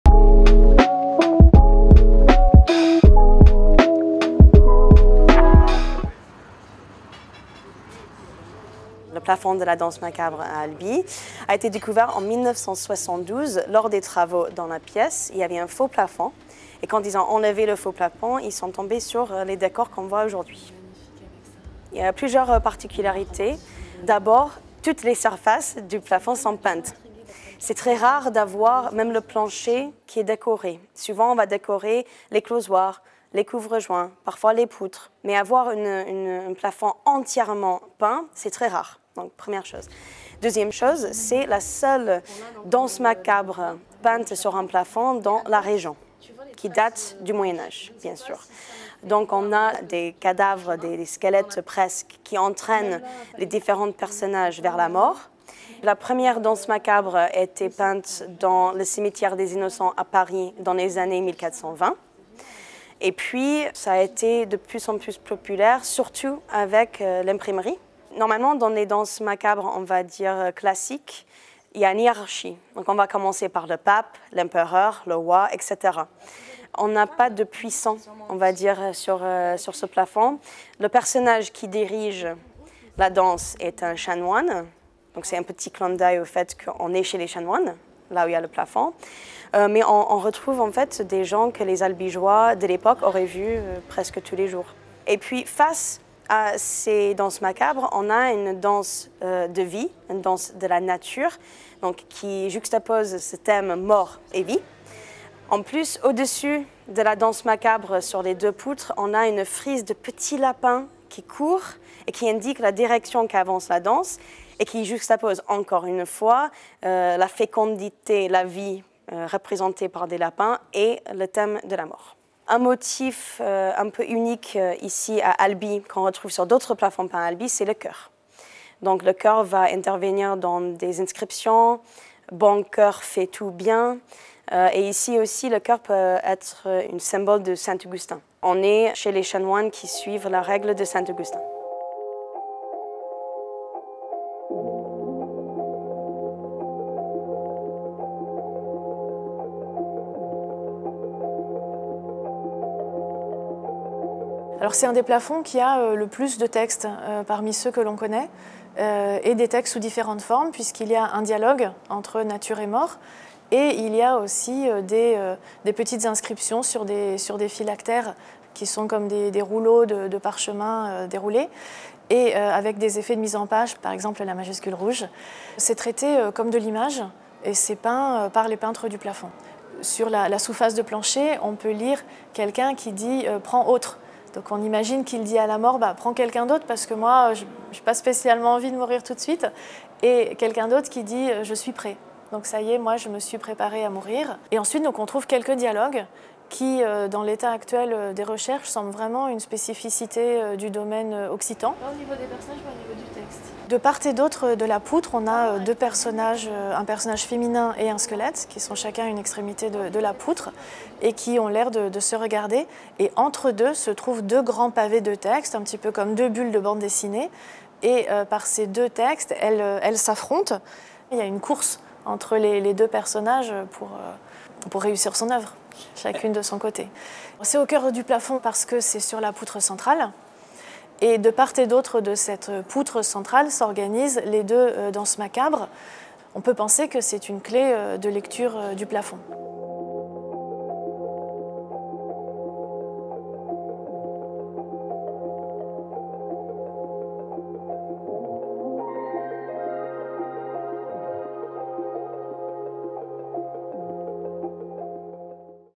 Un plafond peint médiéval à Albi - La maison des Chanoines
Le plafond peint dit « de la danse macabre » à Albi est une merveille du patrimoine médiéval dans la région Occitanie. Dans cette maison de chanoine, des hommes sont entraînés vers la mort par des squelettes dansants. Autour d’eux gravitent animaux, hybrides, danseurs, inscriptions, et armoiries aux vives couleurs.